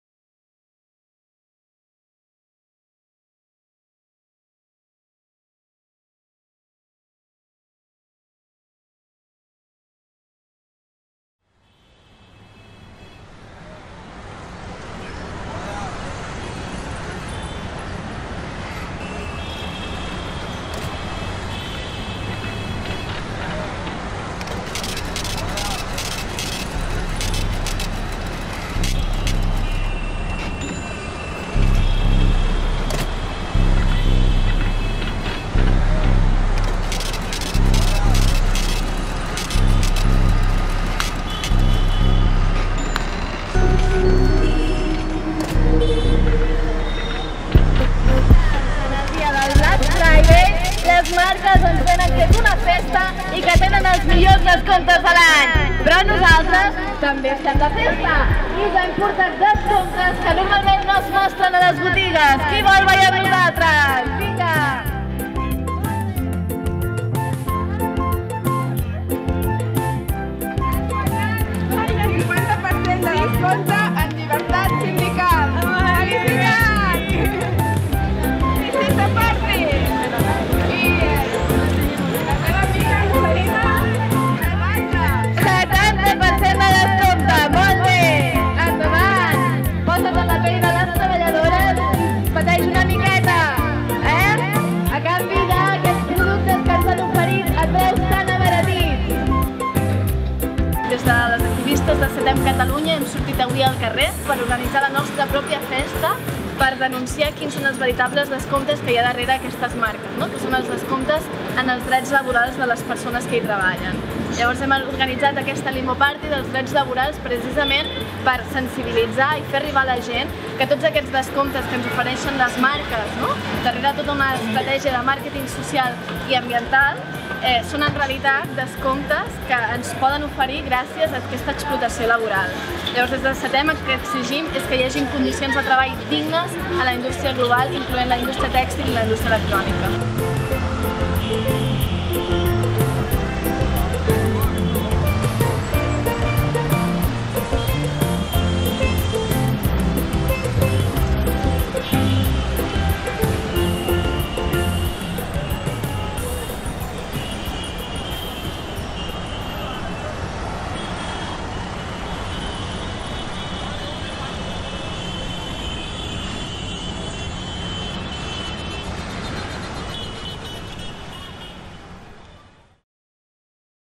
La Limbo Party dels drets laborals: la lluita contra el Black Friday al carrer!
El Black Friday és un esdeveniment impulsat per les empreses transnacionals que simbolitza l’espiral de consumisme capitalista en què vivim, que es manté a través de sistemàtiques violacions dels drets humans de milions de persones.
Les activistes de SETEM Catalunya han sortit al carrer per celebrar la Limbo Party dels drets laborals: les rebaixes en els preus dels productes que comprem són possibles gràcies a les rebaixes en salaris, seguretat o llibertat sindical que pateixen les treballadores.
Lluitem contra les injustícies i l'explotació laboral!